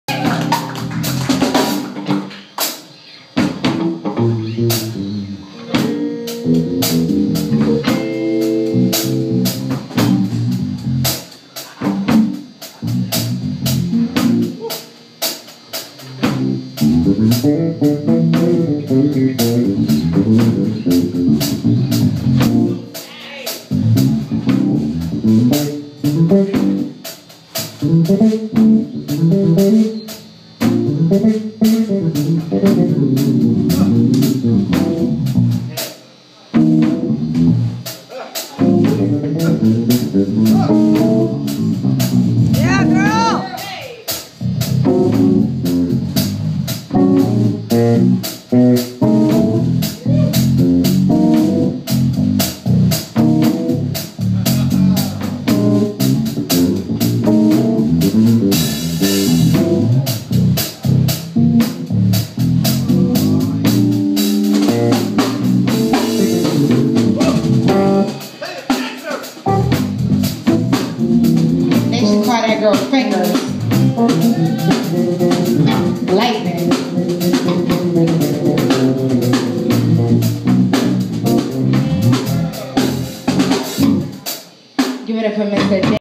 Zuri Appleby takes a bass solo
Zuri Appleby taking a bAss solo in Buffalo,NY at DGBG's